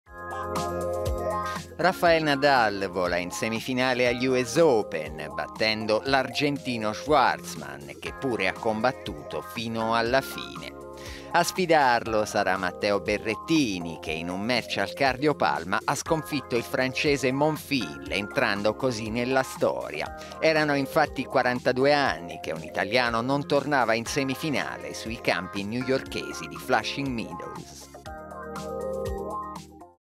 US Open: Berrettini batte Monfils e vola in semifinale contro Nadal
Ancora una partita straordinaria per Matteo Berrettini, che approda in semifinale dopo aver sconfitto il francese Monfils ai quarti in 5 set (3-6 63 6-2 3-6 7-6). Venerdi affronterà Nadal: mai dire mai. Tra le donne, la seconda semifinale sarà tra le outsider Bencic e Andreescu. …